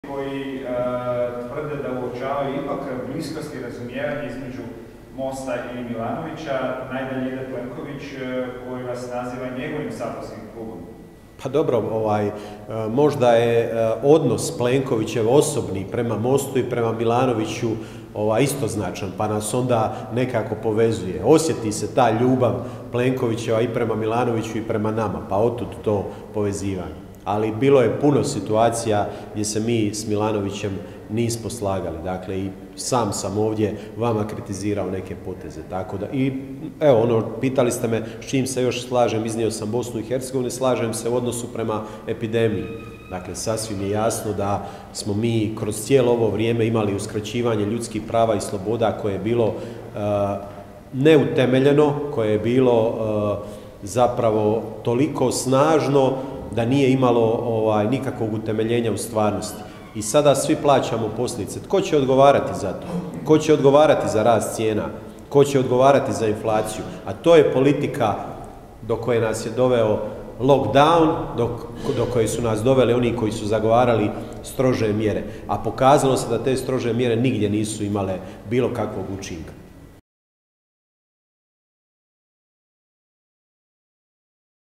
Nikola Grmoja: Osjeti se Plenkovićeva ljubav prema Milanoviću i MOST-u
Saborski zastupnik Mosta Nikola Grmoja na konferenciji za medije održanoj u Saboru govorio je o azumijevanju Mosta i predsjednika Milanovića.